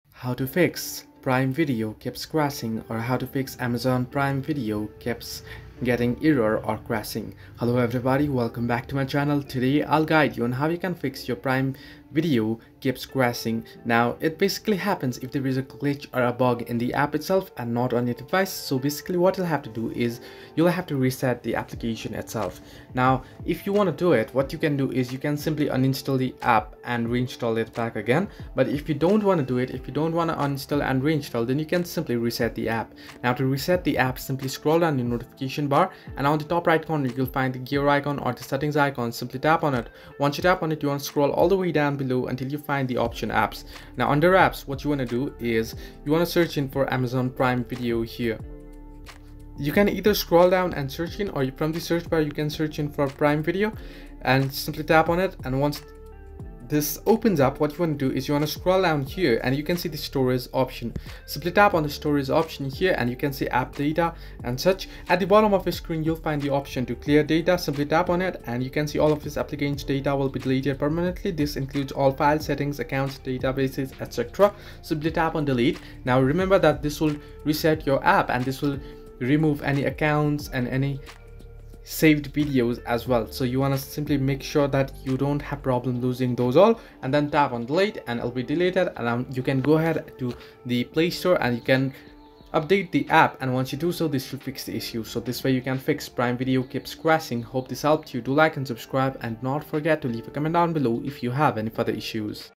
Fix Prime Video Keeps Crashing !
In this tutorial video, I will simply show you how to fix amazon prime video keeps crashing. So just make sure to watch this video till the end.

~ Video Chapters:
0:00 Introduction
0:12 Overview & Reinstall
0:30 Reset App
1:12 Outro